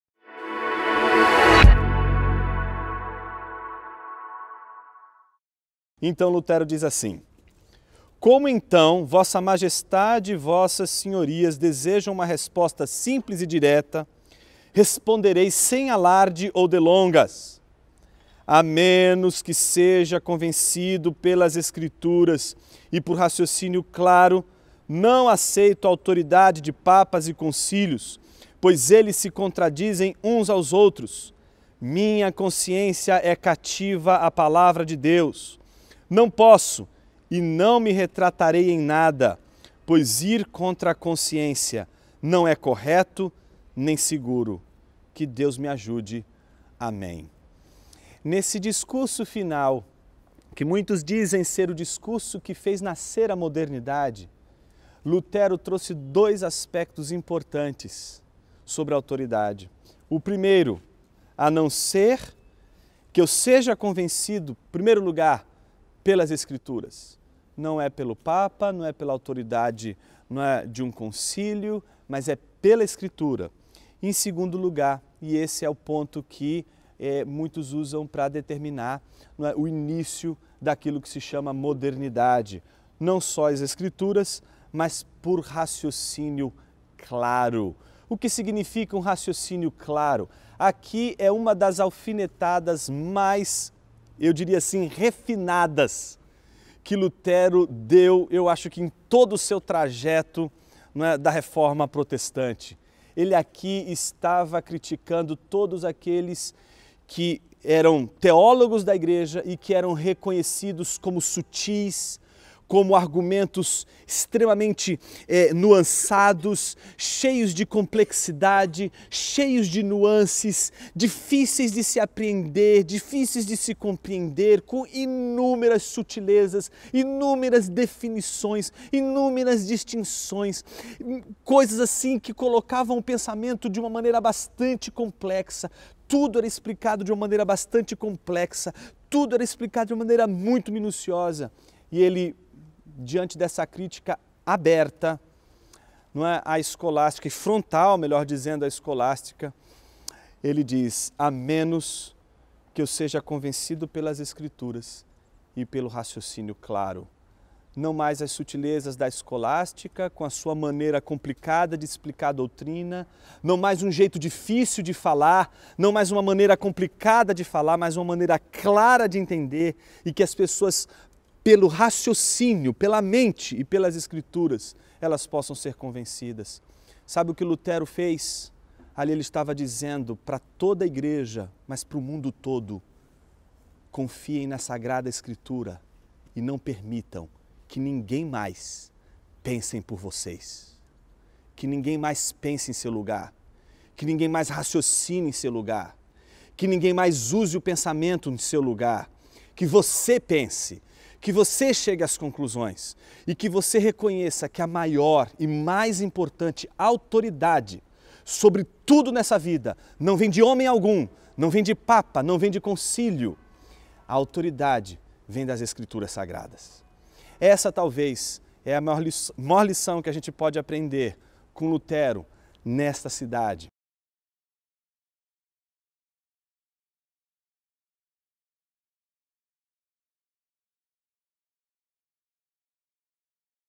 Como a Reforma deu origem a modernidade — Jonas Madureira
Jonas Madureira explica aqui como o discurso de Lutero deu origem a modernidade.

VOLTEMOS AO EVANGELHO